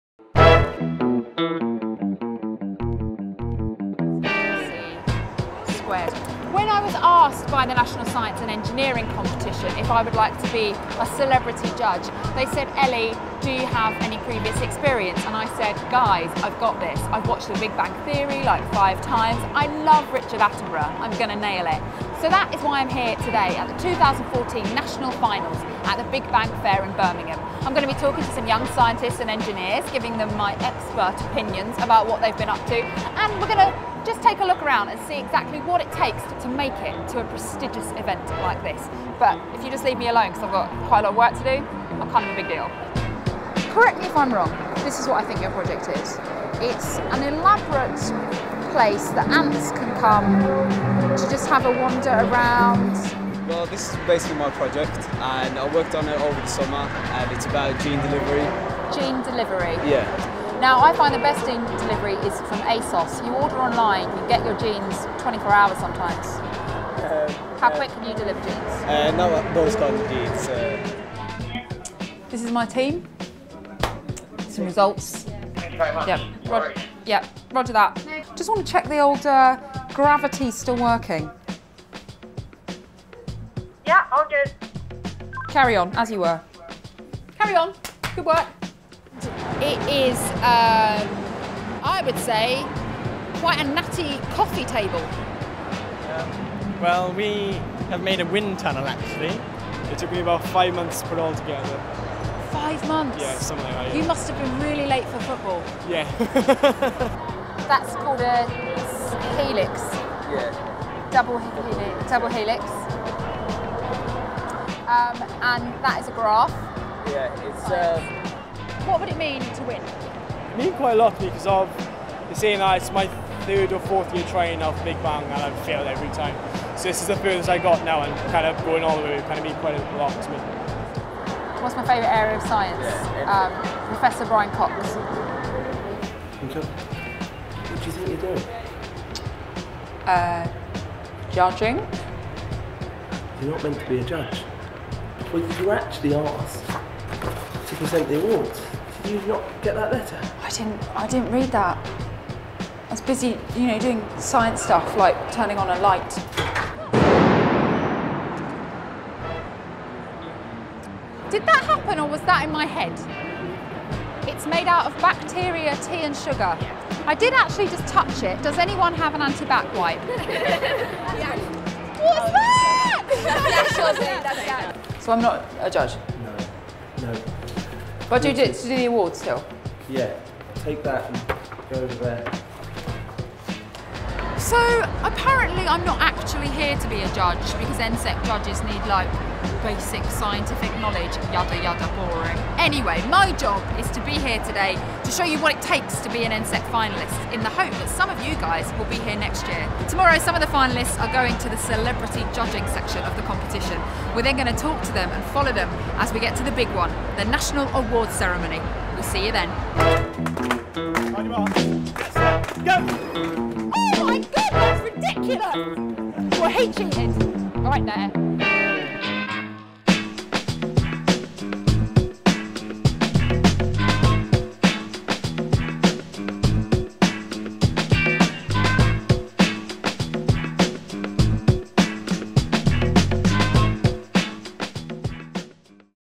Day one at the National Finals 2014 with Ellie Taylor
Ellie Taylor (Snog, Marry, Avoid and Fake Reaction) meets National Science + Engineering Competition finalists at The Big Bang Fair in Birmingham on day one.

There seems to a be a bit of confusion about Ellie's role at the finals - watch how she gets on.